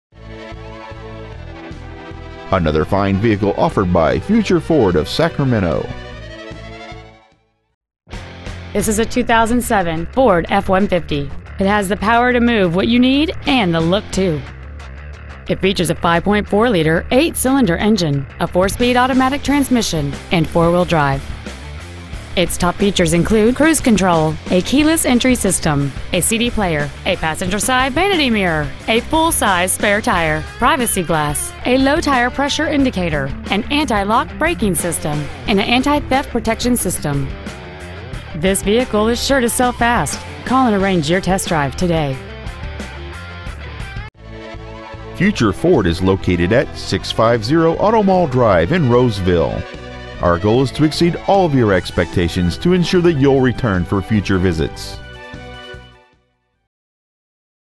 2007 Ford F-150 Citrus Heights CA
Year : 2007

 Make : Ford

 Model : F-150

 Engine : Gas V8 5.4L/330

 Trans . : Automatic

 Exterior : Brown

 Miles : 95,031

 Future Ford

 650 Automall Drive

 Pre-Owned 2007 Ford F-150 Roseville CA